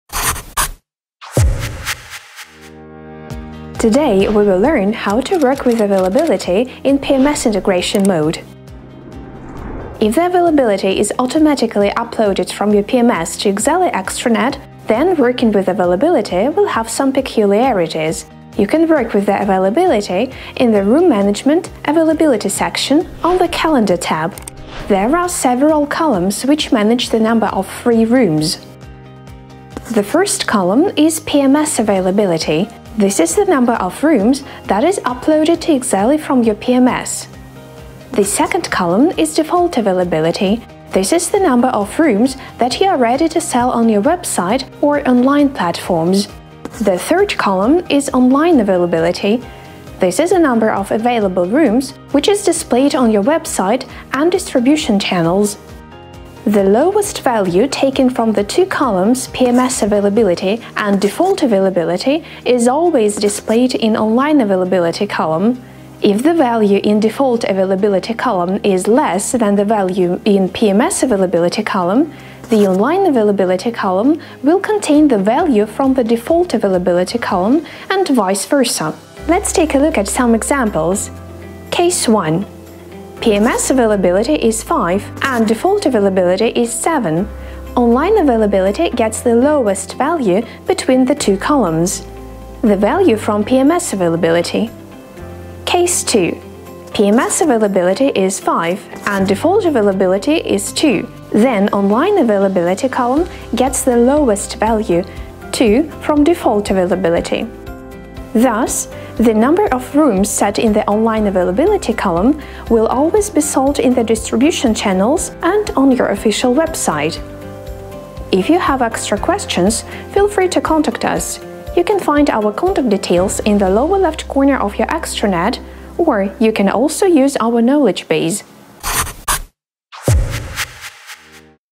How to Work with Availability in PMS Integration Mode - Your Hotel's Guide to Exely Extranet.
Greetings from the Exely Customer Support team! 👋

Managing availability just got easier! In this quick tutorial, you'll learn how to work with room availability while using PMS Integration Mode in the Exely Extranet. Ensure your property’s data stays accurate and synced across all platforms without manual updates.

👉 In this video, you’ll learn how to:
✓ Understand availability sync with your PMS
✓ Adjust availability settings in Exely
✓ Avoid overbookings and mismatches
✓ Maintain real-time room data across channels